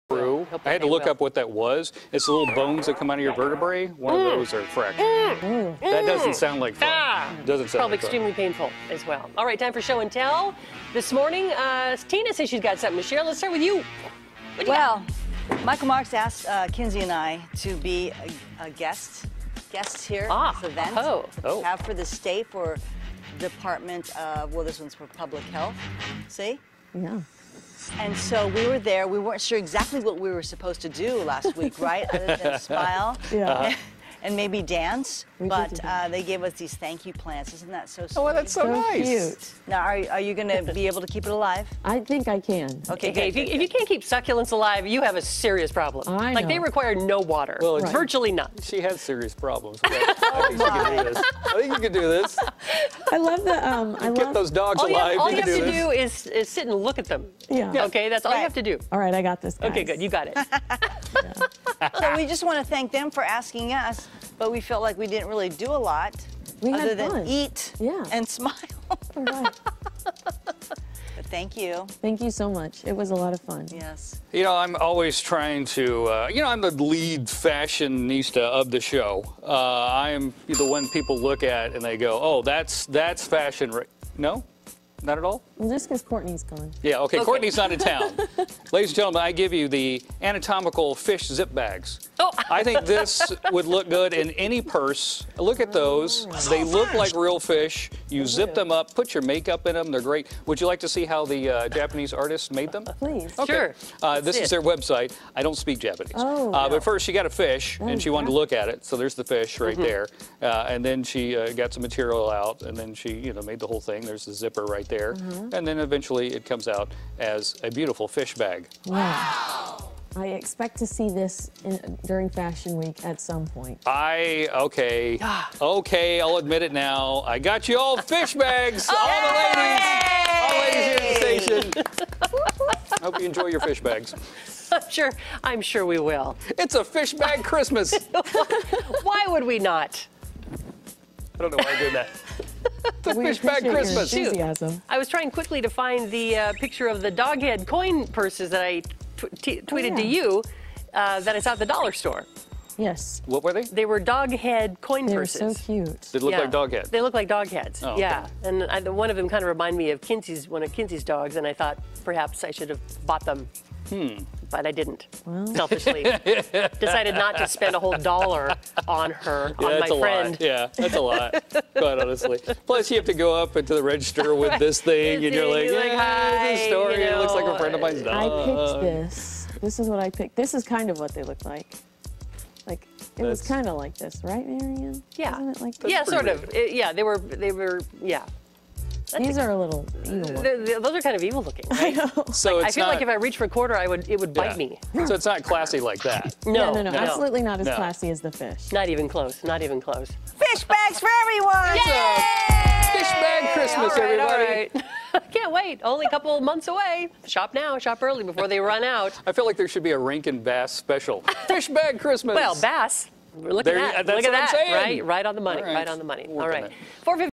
Show & Tell: Fish Zip-Bags
Cody shares a “fishy” new bag trend with us.